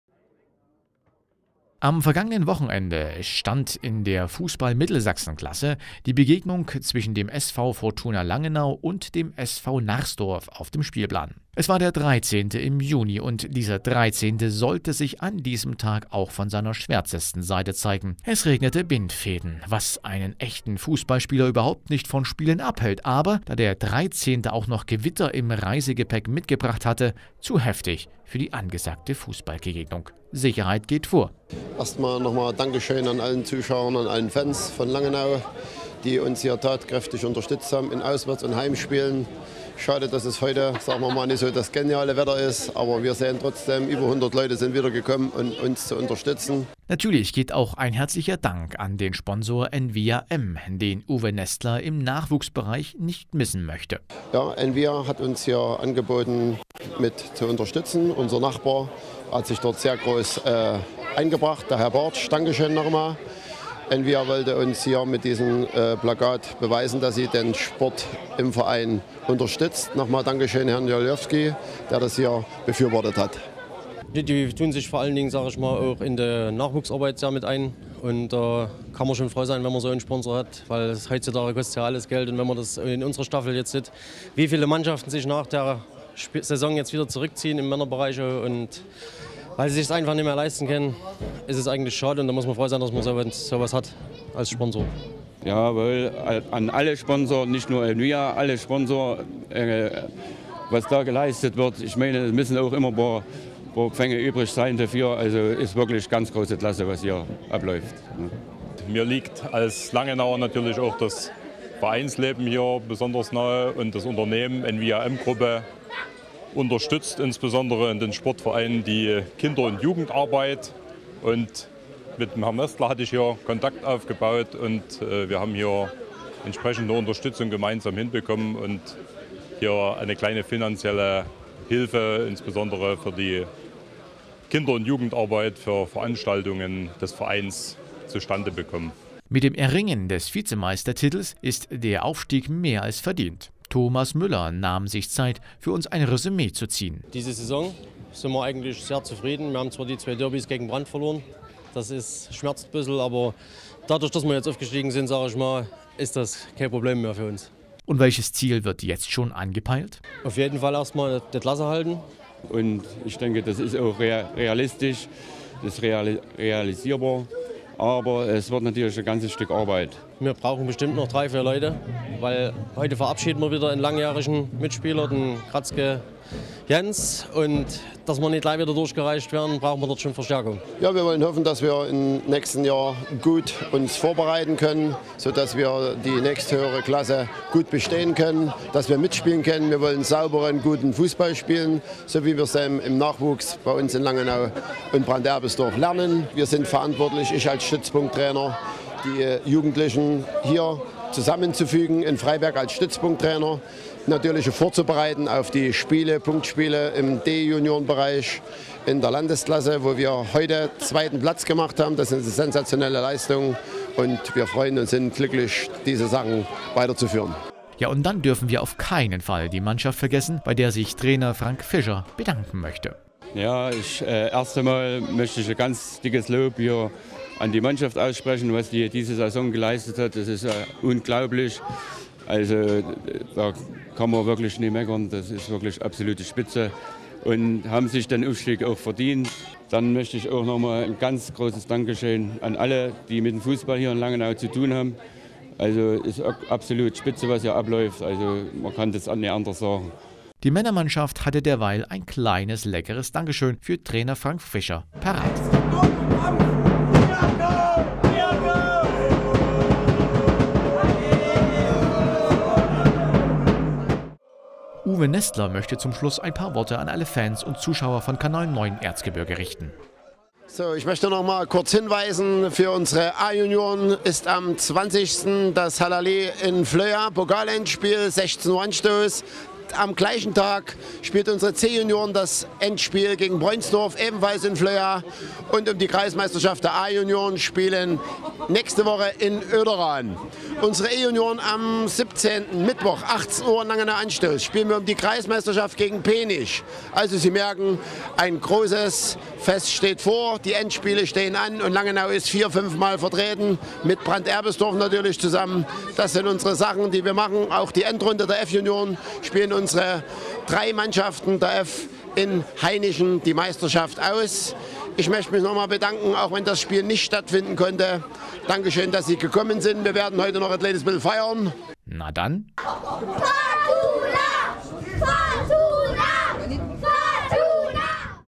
Saisonabschluss beim SV Fortuna Langenau 2015
Eigentlich sollte am letzten Samstag das letzte Heimspiel der Fortuna Langenau angepfiffen werden, aber schwere Gewitter haben das leider verhindert. Dennoch gab es in Langenau allen Grund zu feiern.